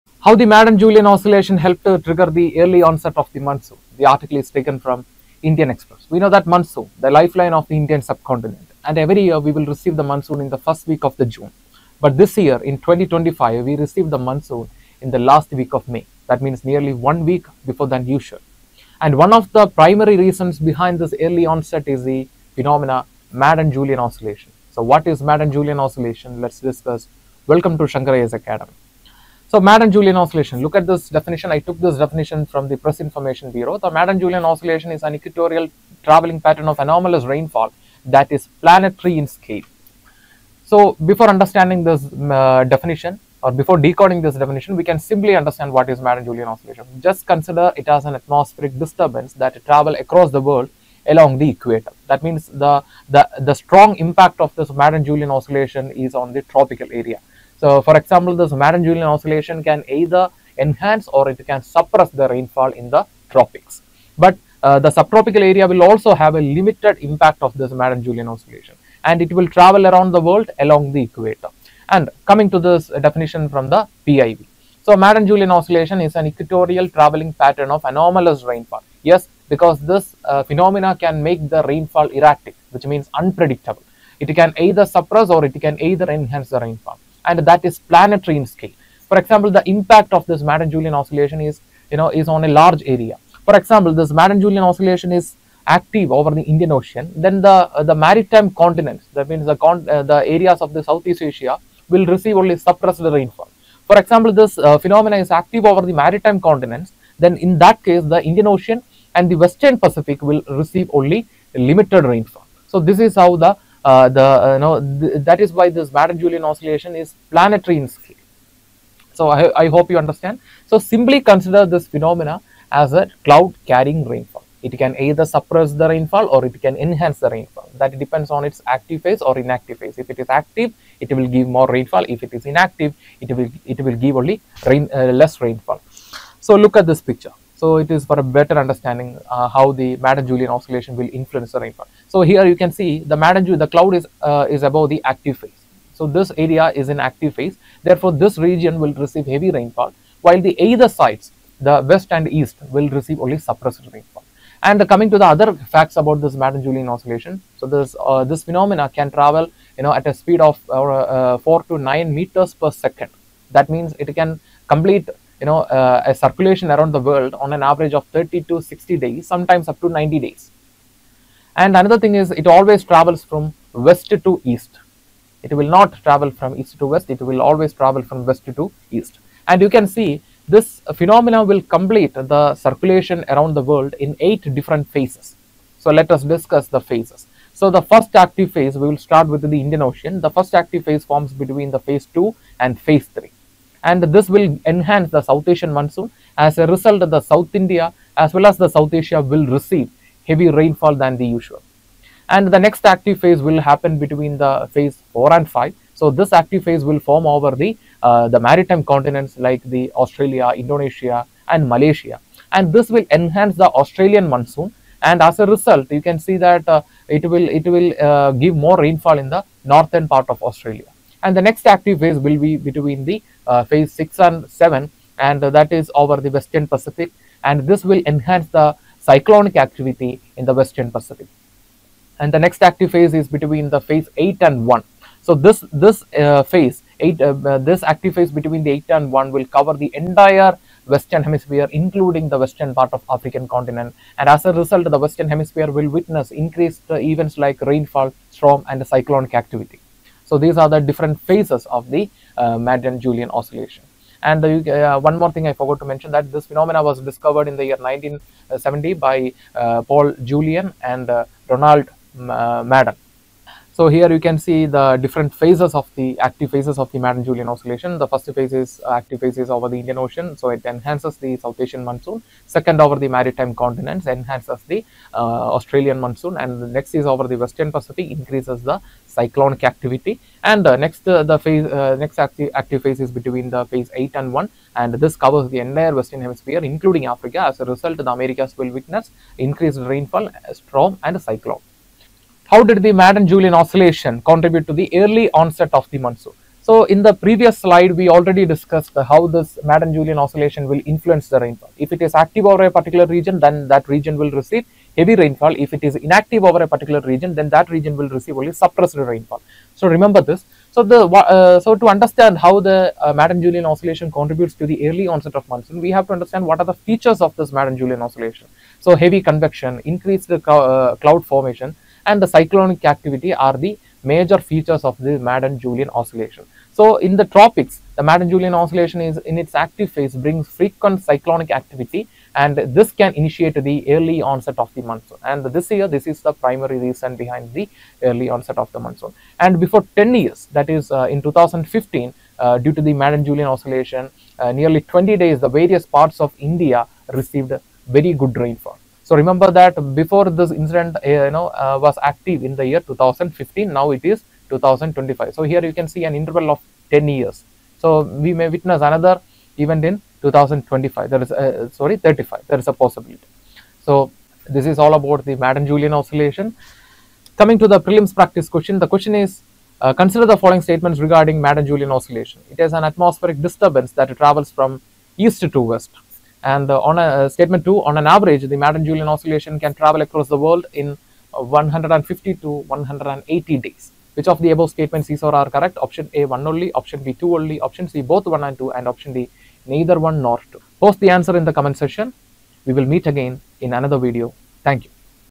Madden-Julian Oscillation Explained | Impact on Indian Monsoon | UPSC current affairs
In this video, we explain the Madden-Julian Oscillation (MJO) — a crucial atmospheric phenomenon that significantly impacts the Indian monsoon, cyclone formation, and global weather patterns.
Understand what MJO is, how it moves, and why it's important for UPSC Prelims and Mains — especially in Geography, Environment, and Current Affairs sections.